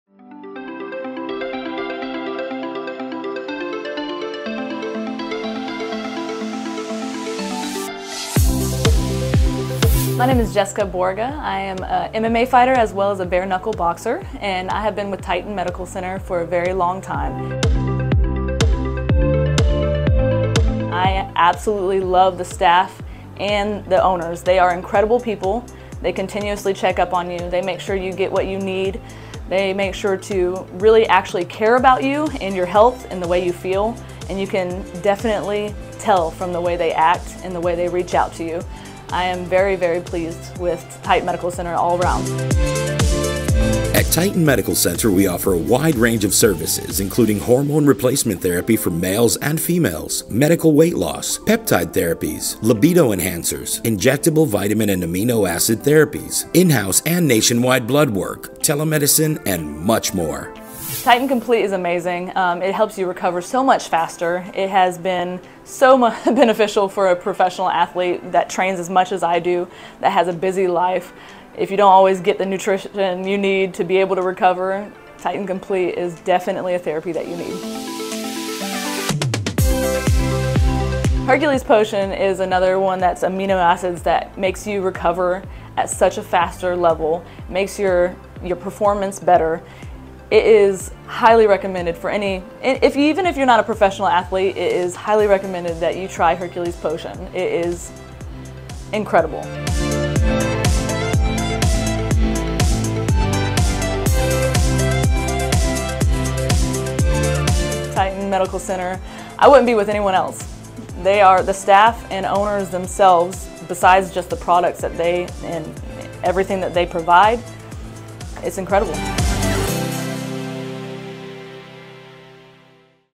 Titan Patient Testimonial by Jessica!
Titan patient Jessica talks about her experience with Titan therapies & service!